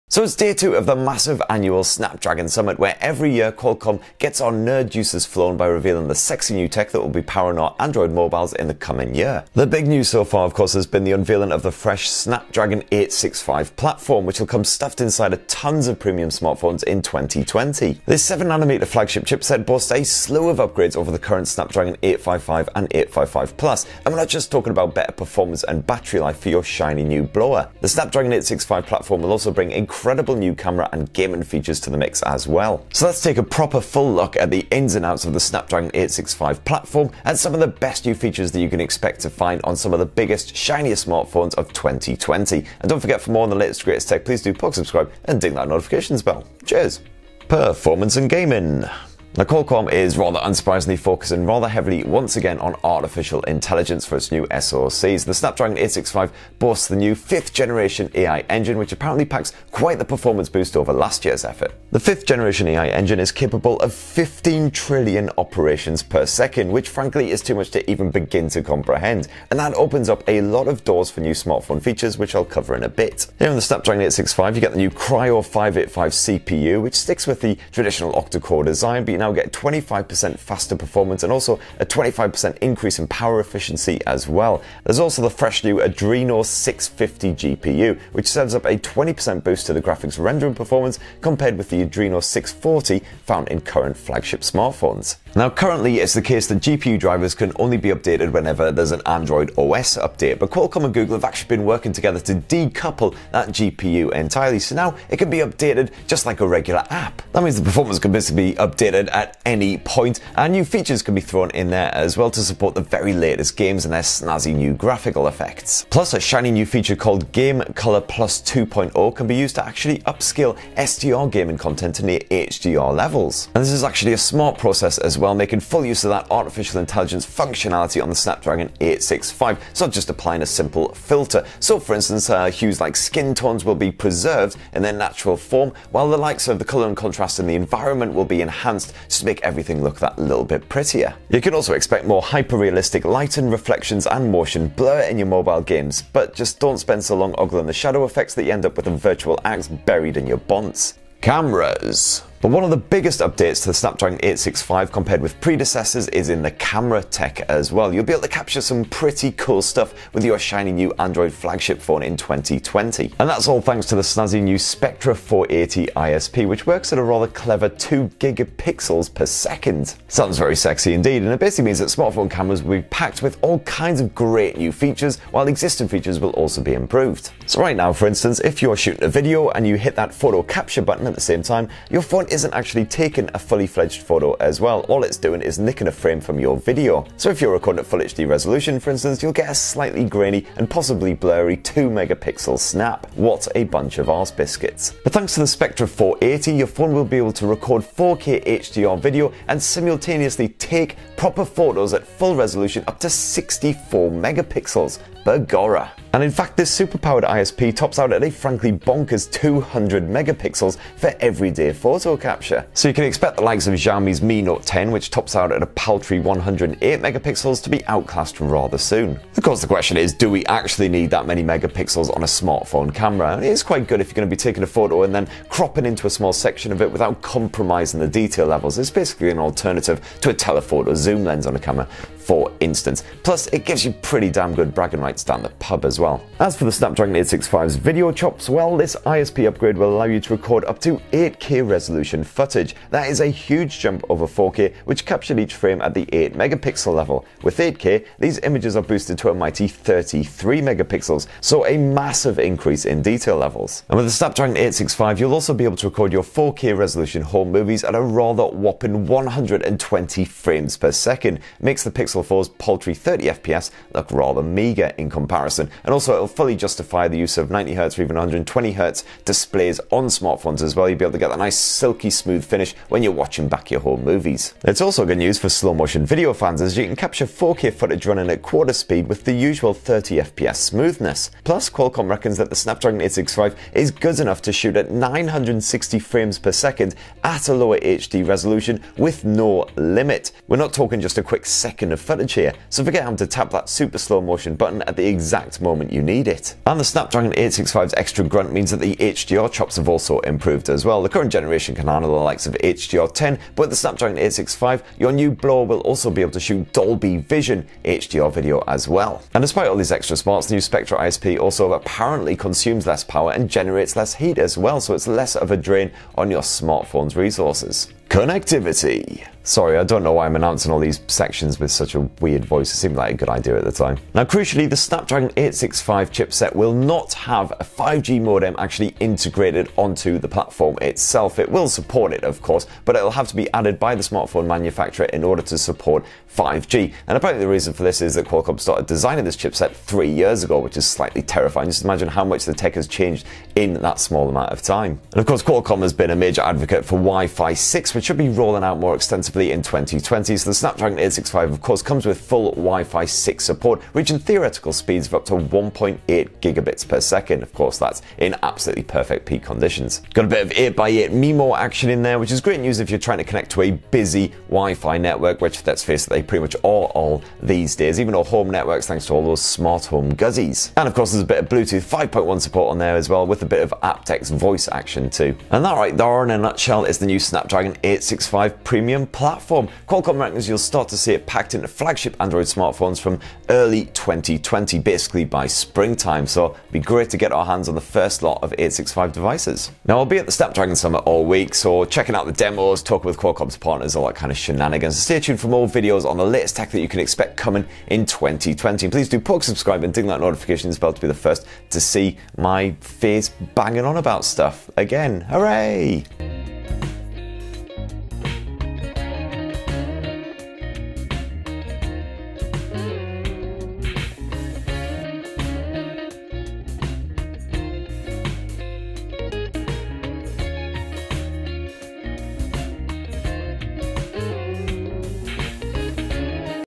Snapdragon 865 In Full | 200MP Photos, PC-Level Gaming & More!
Qualcomm has officially launched the Snapdragon 865 SoC, and teased us with all kinds of great features that we can expect in our flagship Android smartphones in 2020. From the usual performance and specification upgrades versus the old 855 platform, to incredible new camera and gaming features, it's looking like next year will be a massive one for mobile.

Let's review the specs. First up, the new Kryo 585 CPU offers a 25% boost to performance and power efficiency, while the Adreno 650 GPU can produce PC-level gaming experiences on your Android phone. Incredible real-time lighting and reflections, plus upscaling to HDR quality graphics.

But the Snapdragon 865's biggest upgrade compared with rivals is Qualcomm's Spectra 480 ISP. This supports photo capture up to 200MP, 4K video recording at a mighty 120FPS, and sustained 960FPS shooting for slow motion goodness. You'll also be able to take photos and shoot video simultaneously for the first time.

Note that the SD 865 won't come with an integrated 5G modem, although it's of course all setup to work with Qualcomm's existing mobile modems.